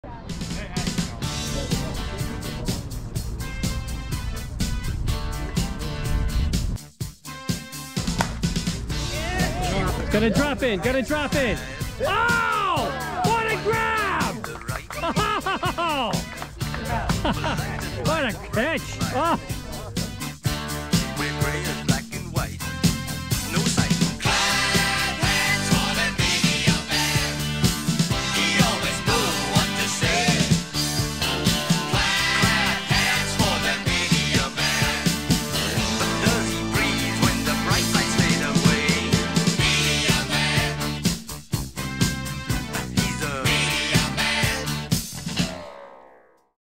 Gold Glove Catch! - May 14, 2024
Chris "Media Man" Pope gives it his all on this play.